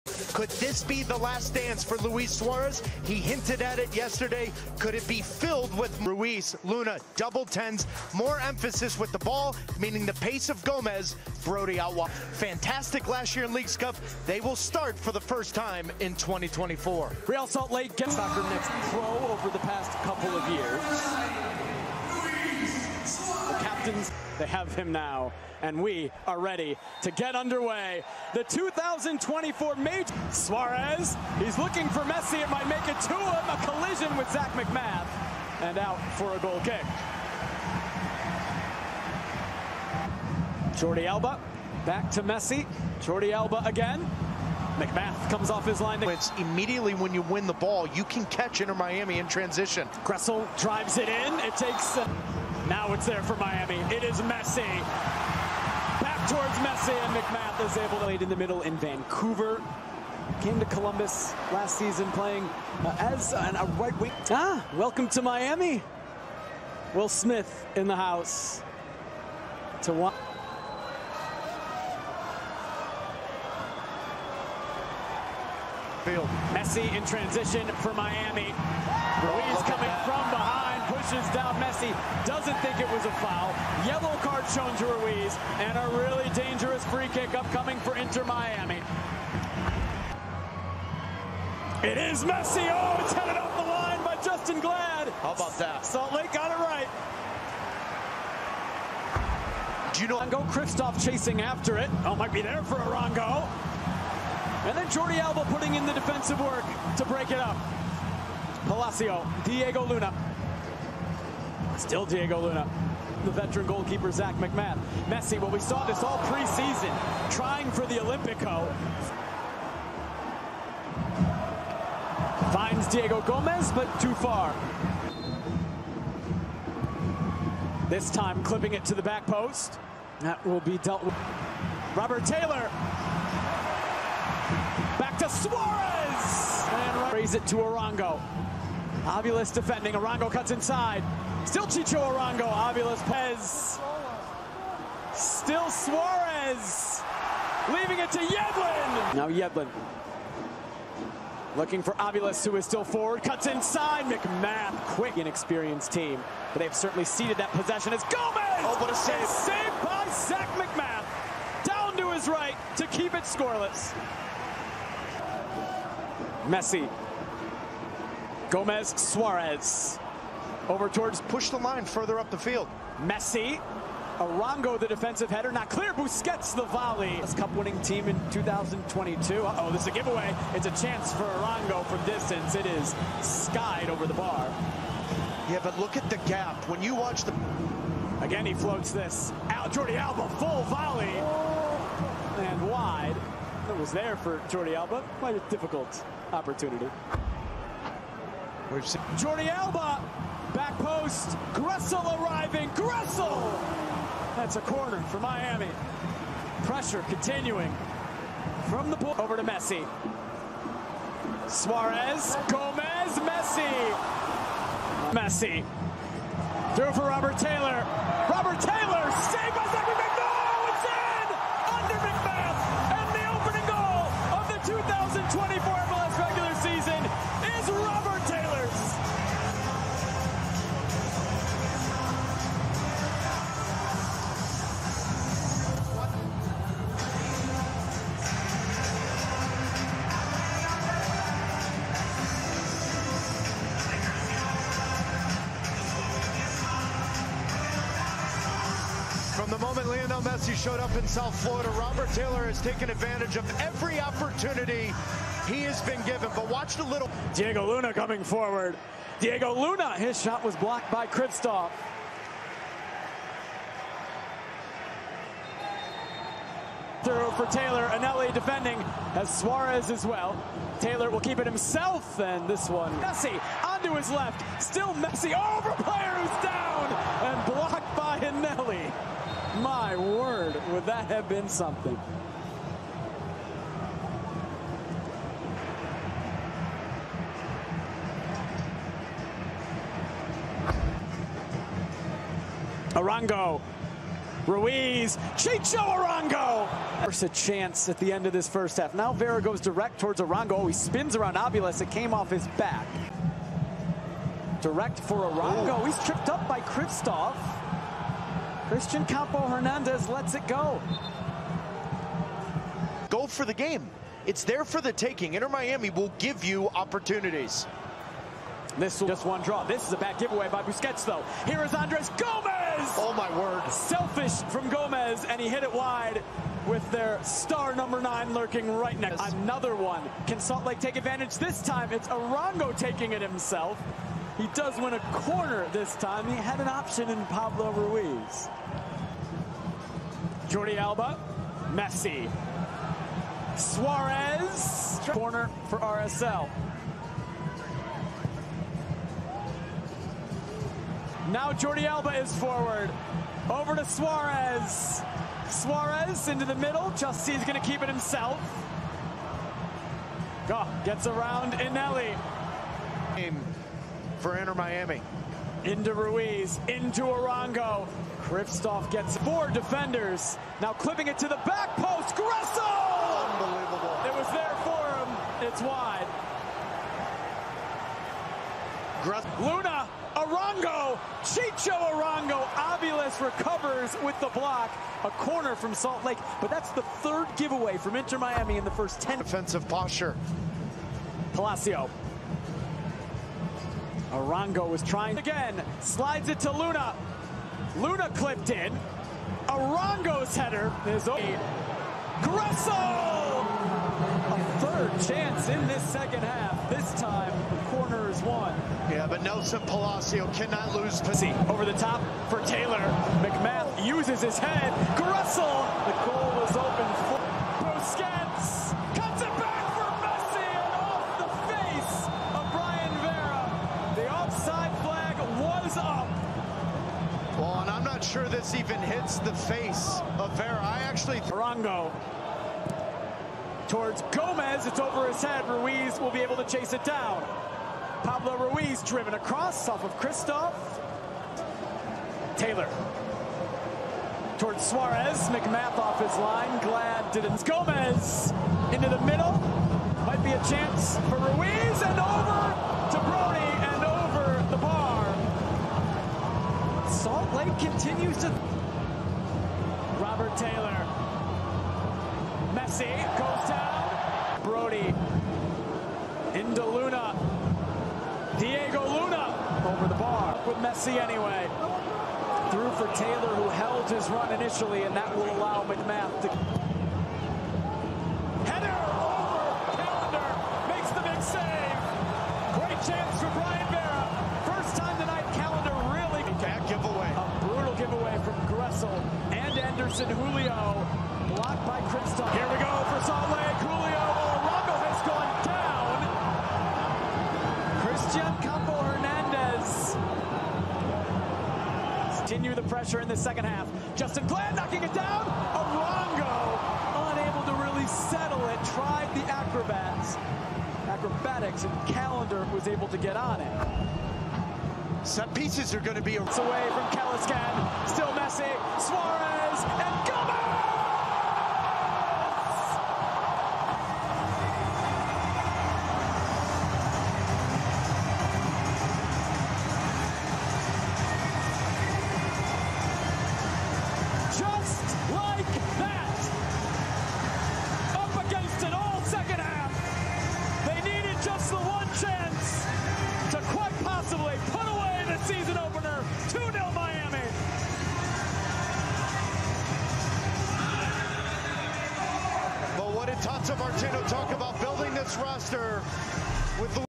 Inter Miami vs Real Salt Lake 2-0 Highlights & Goals | LIONEL MESSI 1A
Goals: Robert Taylor (Lionel Messi 39'), Diego Gomez (Luis Suarez 83')